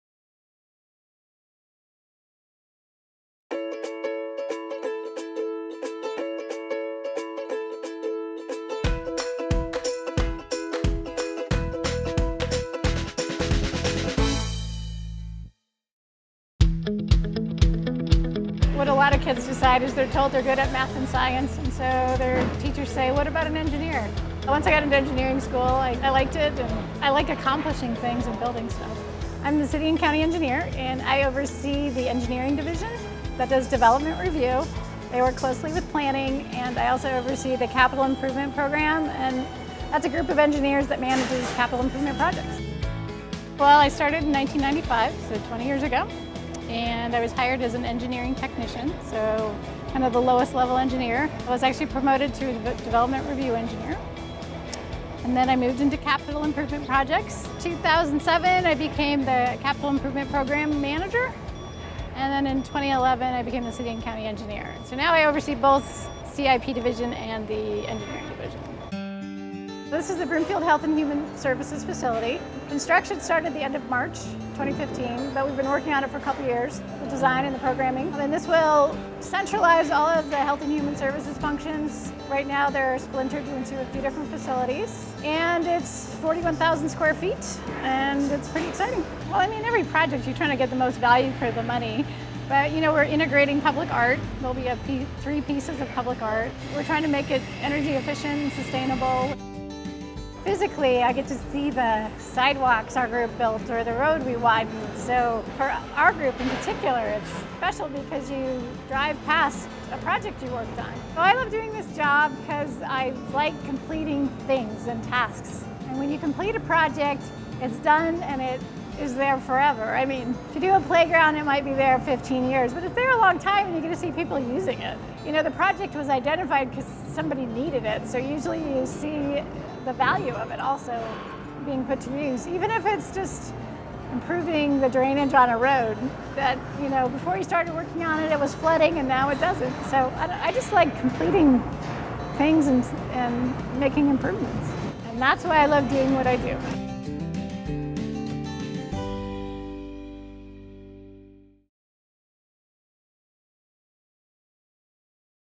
Broomfield Engineer Katie Allen on Why She Loves Broomfield
Hear from City and County of Broomfield Engineer, Katie Allen on why she loves working for the City and County of Broomfield.